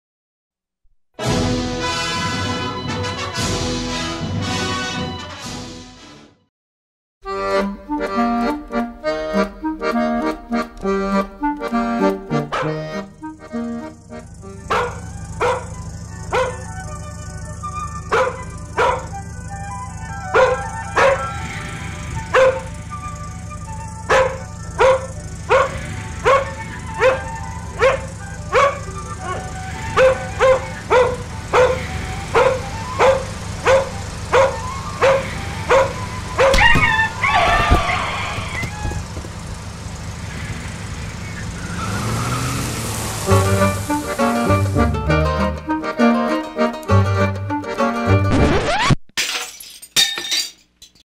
The Angerman Chronicles: Episode 4
Angerman encounters a barky dog on his way to work.